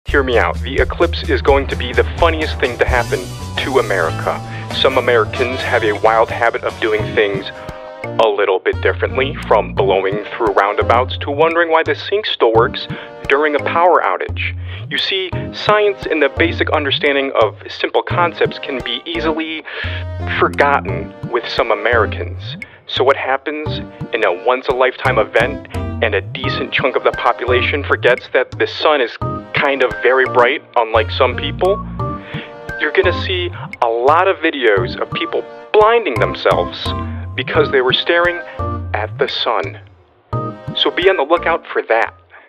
DON'T STARE AT THE ECLISPE
Staring at the Sun is bad, but do other people know this?

This Eclipse is going to show us just who paid attention in class when the teacher told us not to stare into the Sun.